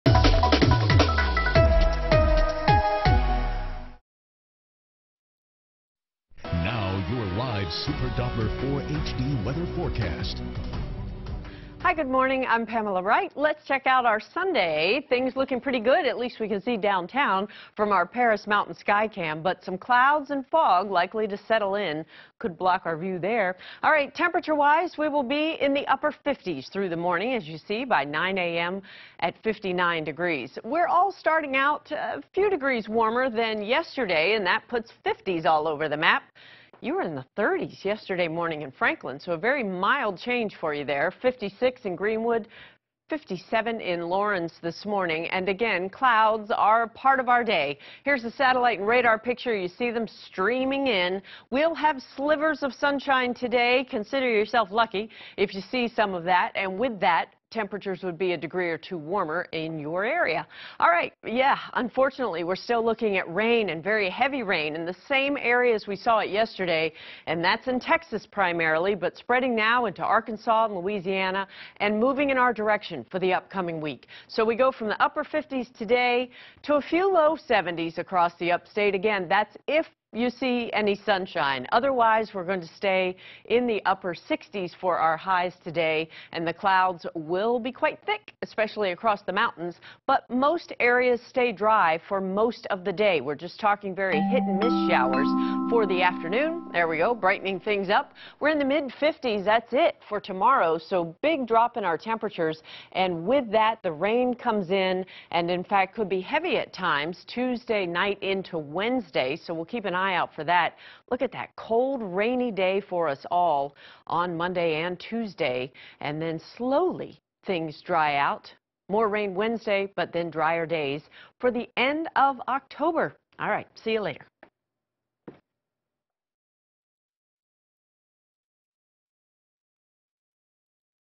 Cloudy & Breezy 10-25
Isolated showers
Highs Upstate 69 WNC 66